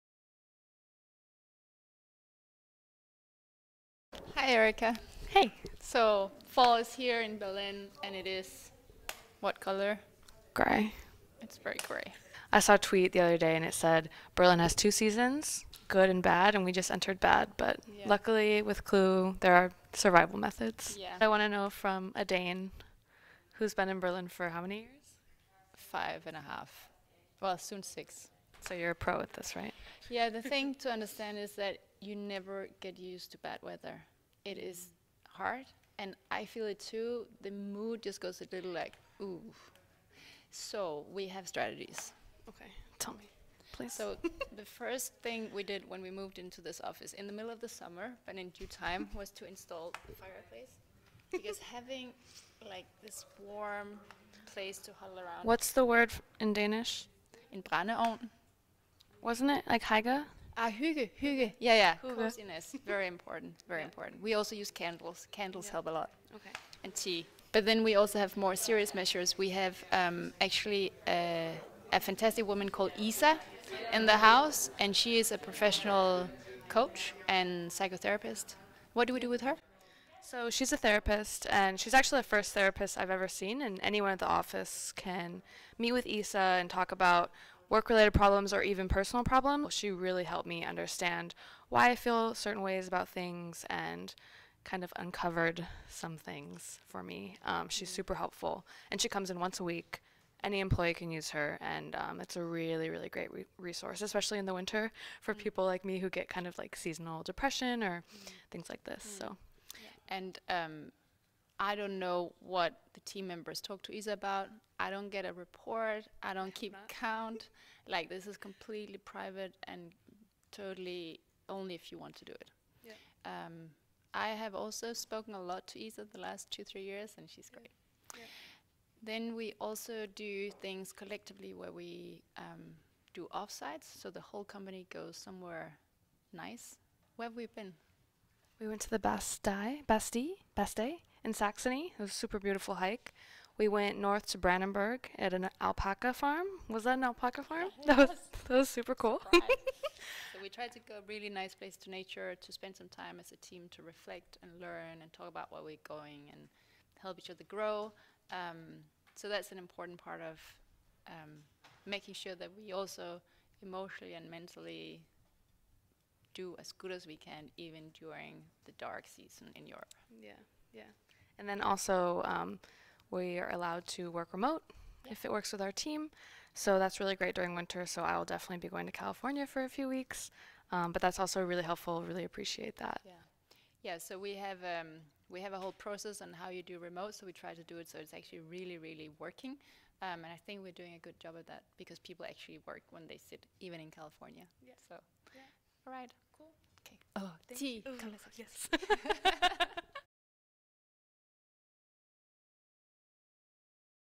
Ida Tin: How Clue survives Berlin winter
In this week's vlog, Ida gives Erica, our community manager from California, some tips on surviving the grey of Berlin winter.